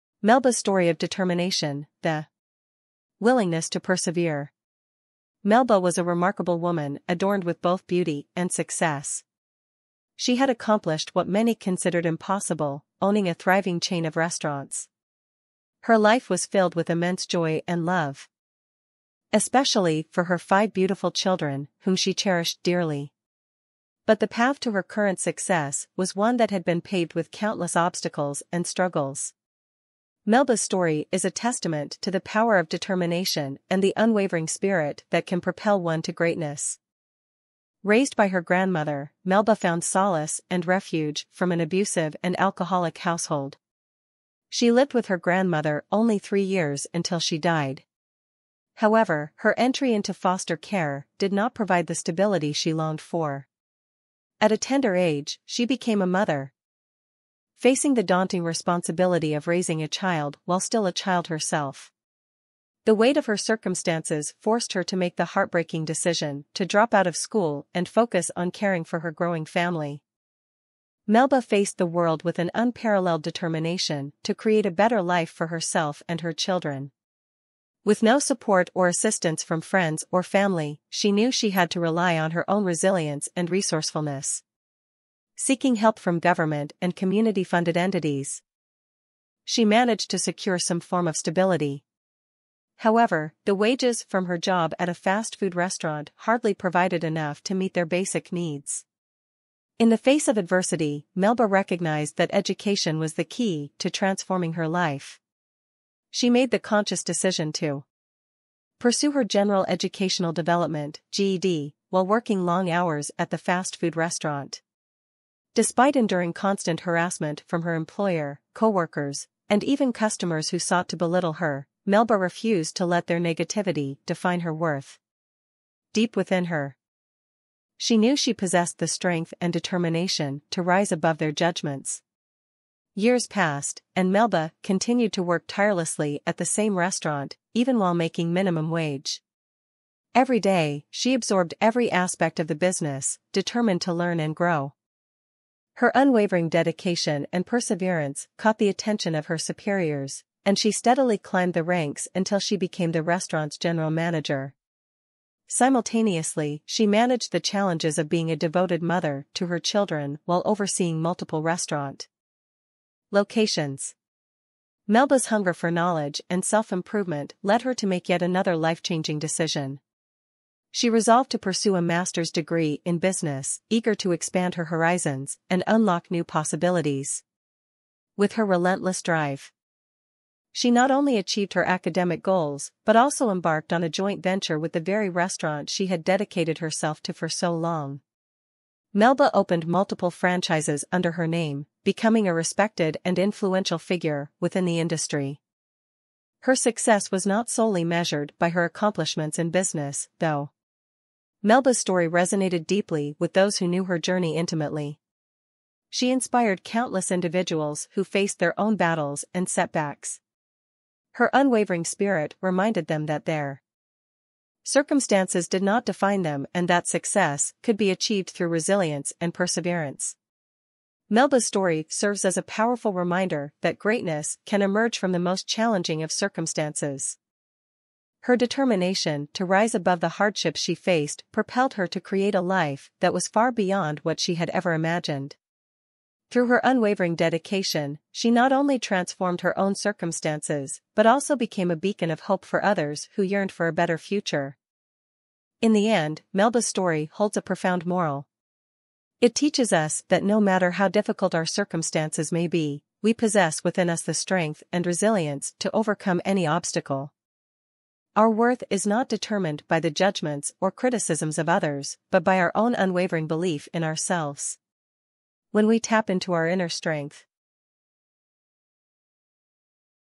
Melba's Story of Determination: The willingness to Persevere
By embodying determination and perseverance, individuals are more likely to overcome obstacles, reach their goals, and fulfill their aspirations. These qualities are not only vital in personal endeavors but also in various aspects of life, such as education, career, relationships, and personal development. Through determination and perseverance, individuals can transform challenges into opportunities and realize their full potential. determination goals overcomingobstacles shortstory life inspire inspirational motivation motivated success fastfood #